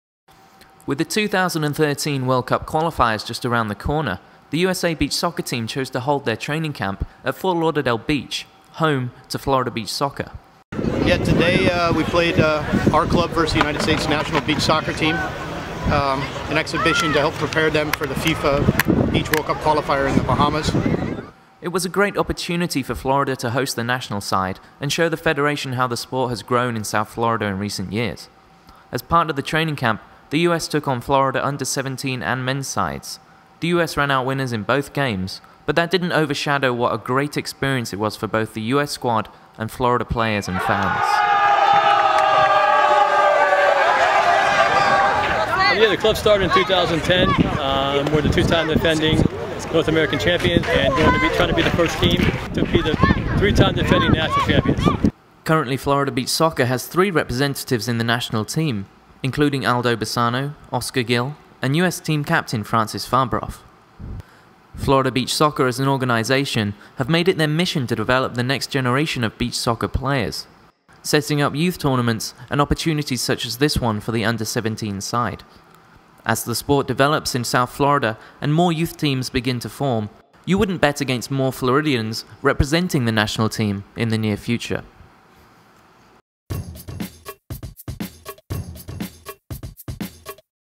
USA train with Florida Beach Soccer
USA prepared for the 2013 World Cup Qualifiers with warm up matches against FBS U17 and FBS FC in Fort Lauderdale beach, Florida.